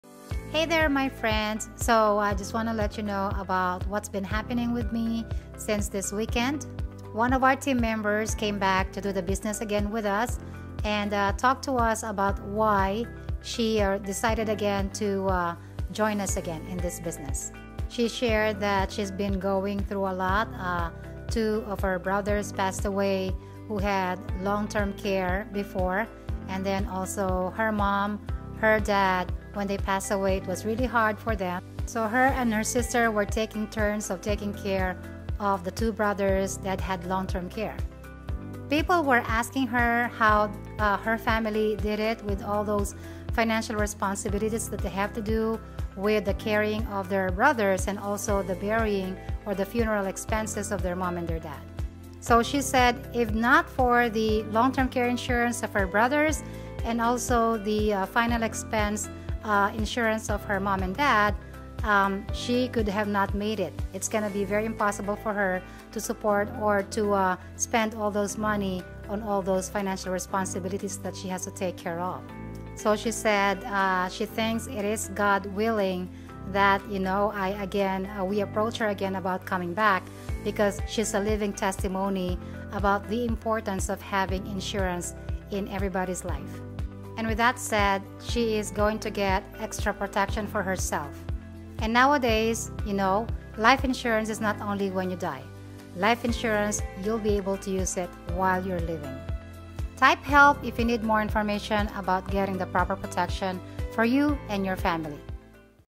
How Life Insurance Saved a Family
Most people nowadays don’t understand the importance of having life insurance or long term care insurance until something happens.

This testimony from my friend is a perfect example of why getting life and long term care insurance is a priority.